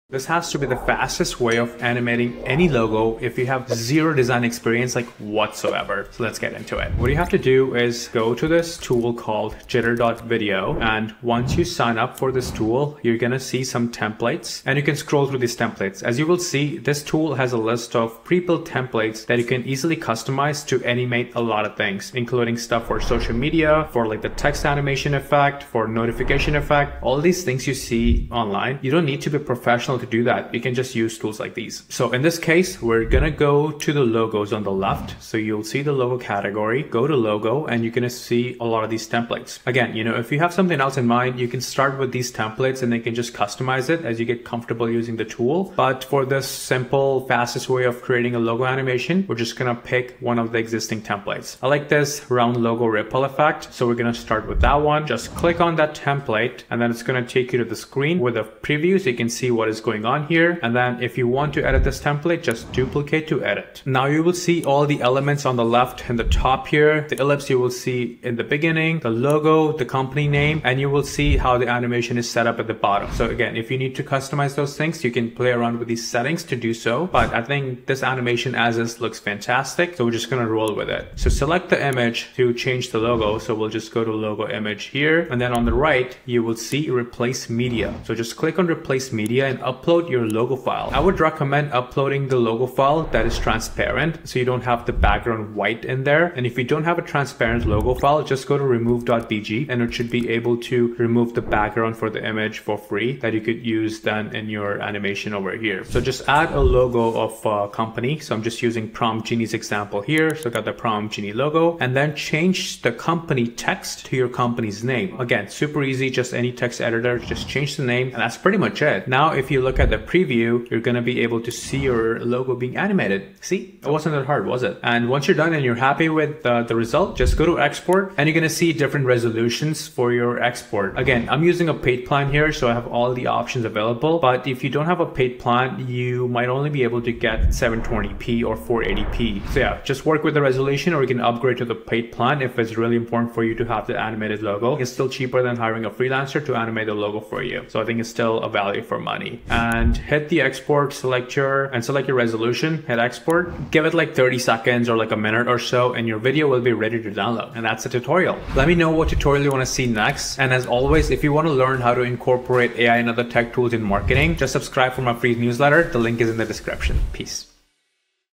✨How To Animate A Logo For Your Business In Only 2 Minutes For Free | Jitter Tutorial
If you're looking to animate your logo for your business or just want to make your current logo look more dynamic, then this is the video for you! I'll show you how to create a logo animation in just minutes, and you'll be able to apply it to any website or project you want!

00:00 The fastest way to animate any logo
00:07 Go to Jitter.video
00:32 Go to Logos
00:55 Choose your template in Jitter
01:21 Customize the template in Jitter
02:05 Export your logo
02:38 What you should do next